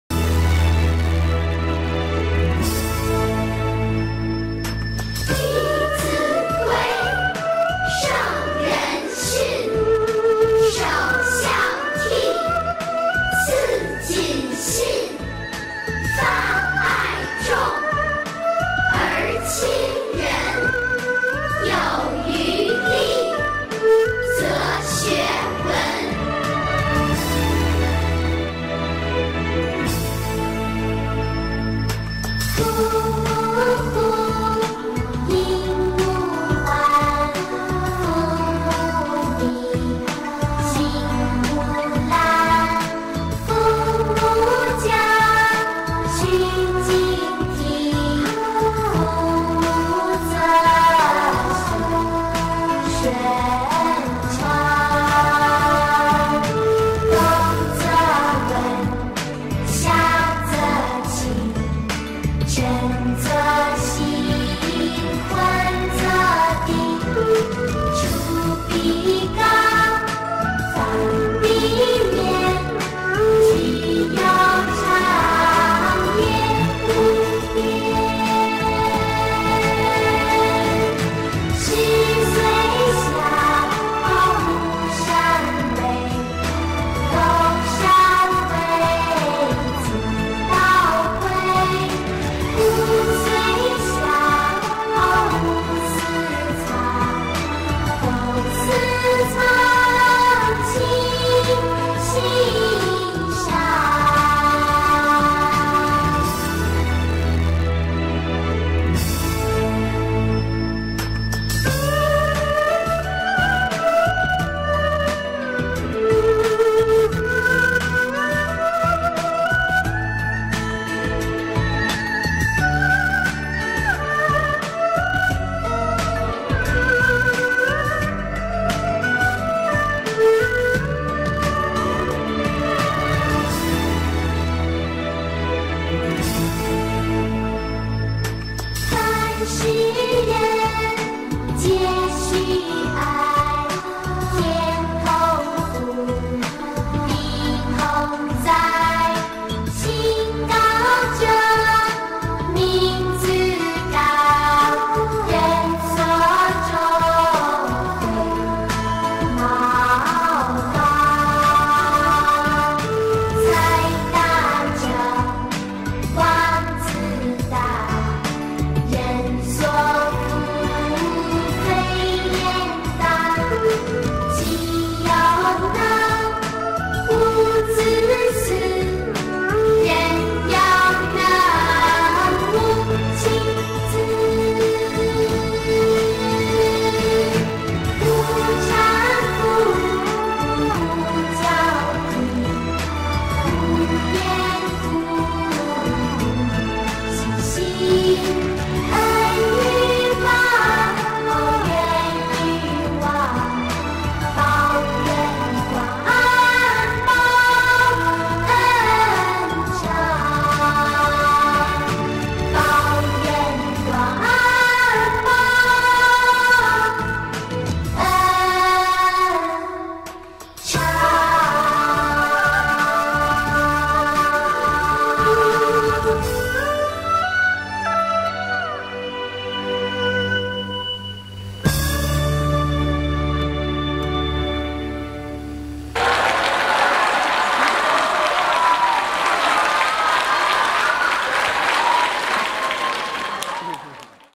Disciple Gauge
Two of our 5 to 9 y. o. Tinies classes dance together.